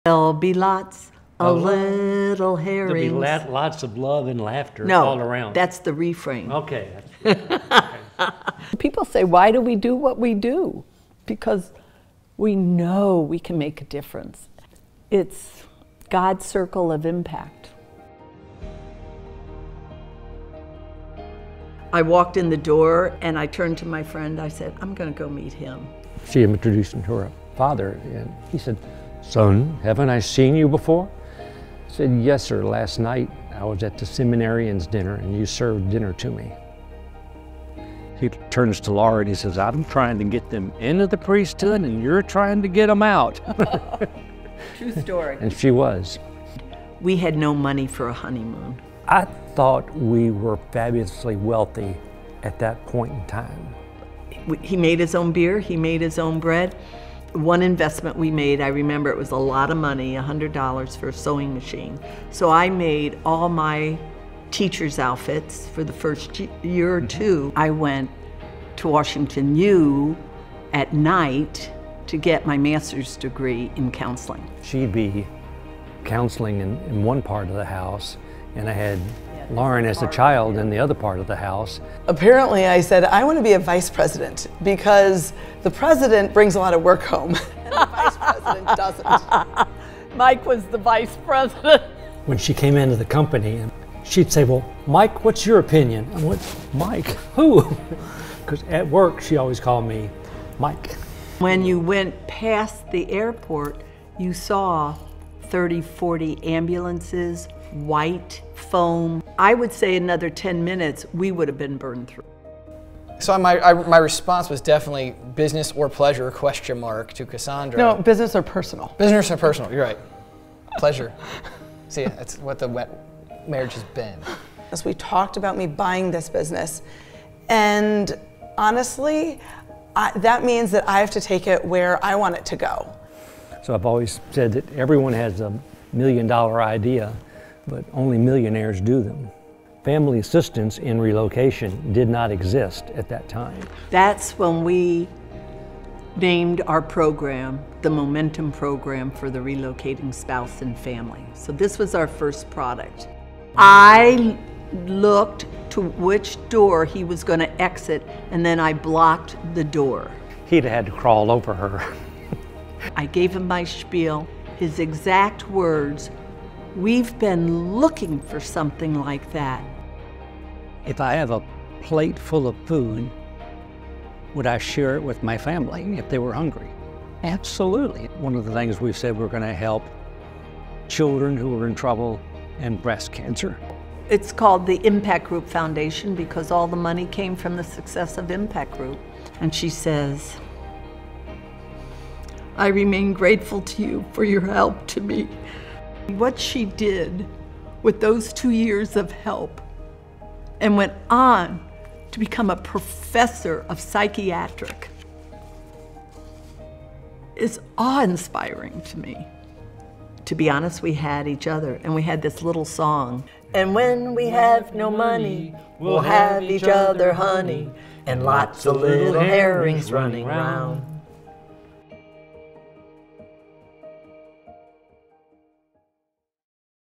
The Herring Legacy Project Trailer
The Herring Family wanted a Legacy Film that would preserve the stories that shaped their family and their business. This is a 1-minute trailer of the film they intend to impact future generations.